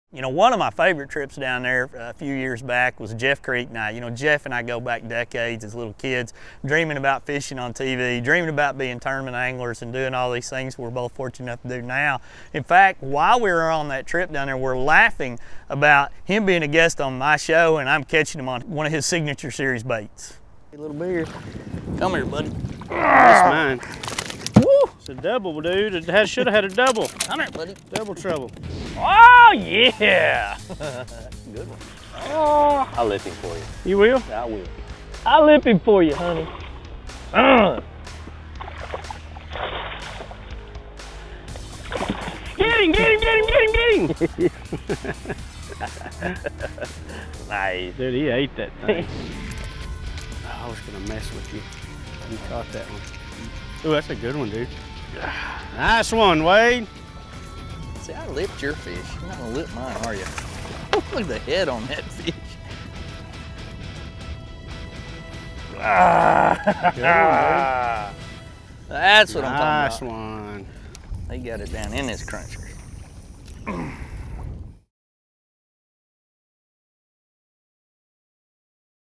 Big Worms Bass Fishing on Falcon Lake with Wade's Middleton and Jeff Kriet fishing
A  lookback at a fun summertime fishing trip with Jeff Kriet and Wade Middleton fishing on Falcon Lake in summer of 2012 fishing Big Worms off shore in the heat of the summer

Falcon International Reservoir (Spanish: Embalse Internacional Falcón), commonly called Falcon Lake, is a reservoir on the Rio Grande 40 miles (64 km) southeast of Laredo, Texas, United States, and Nuevo Laredo, Tamaulipas, Mexico. The huge lake is bounded by Starr and Zapata Counties on the Texas side of the international border and the municipality and city of Nueva Ciudad Guerrero on the Tamaulipas side. The reservoir was formed by the construction of the Falcon Dam to provide water conservation, irrigation, flood control, and hydroelectricity to the area.